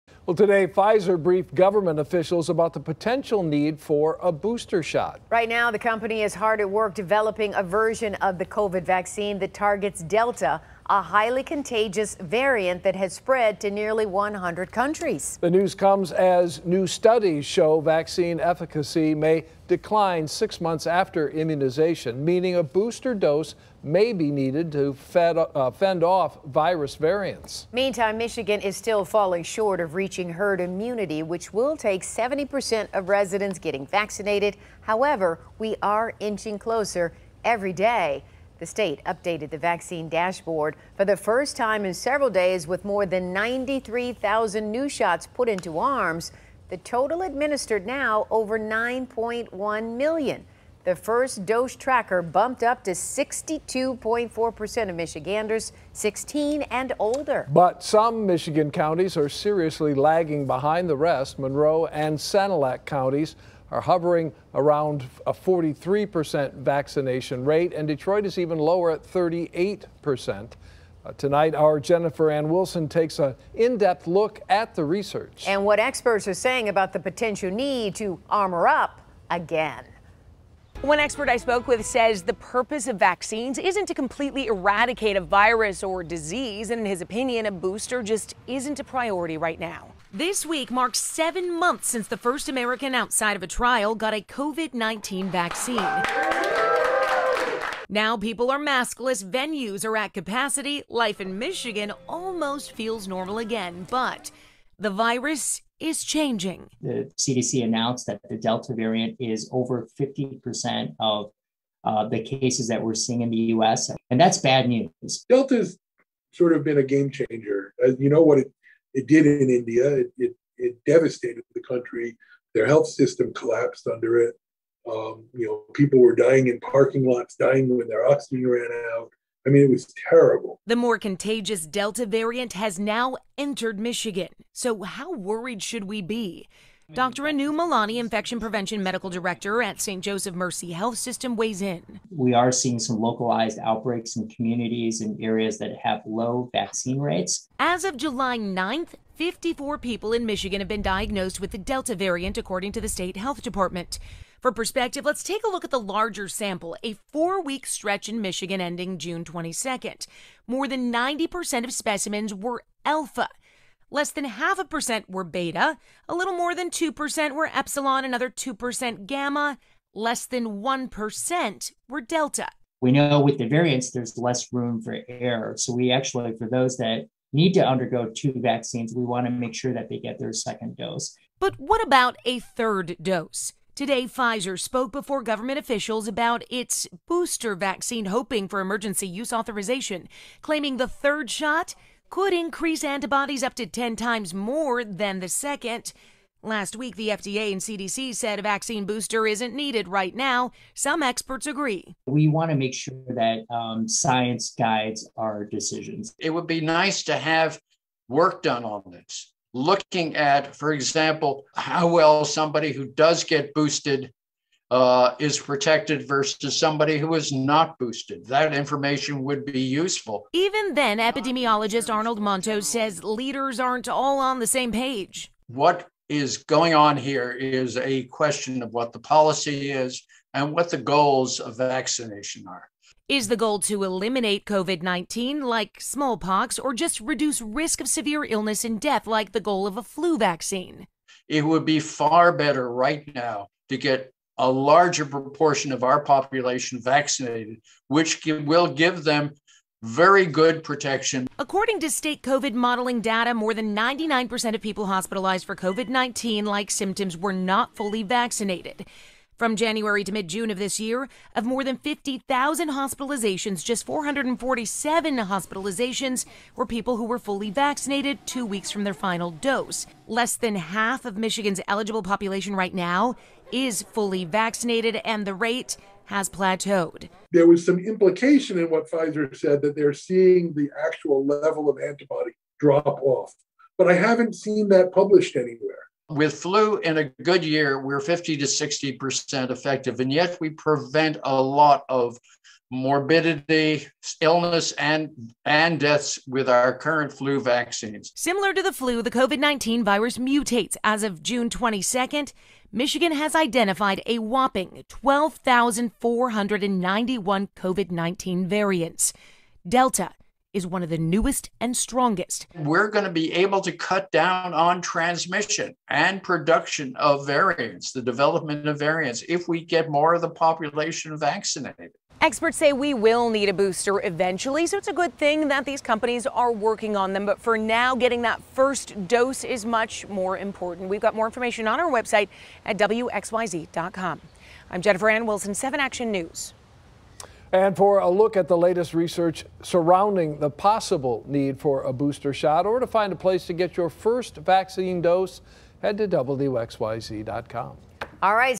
As the Delta COVID-19 variant continues to spread, will we need a vaccine booster?
Hundreds of millions of Americans have rolled up their sleeves for the COVID-19 vaccine.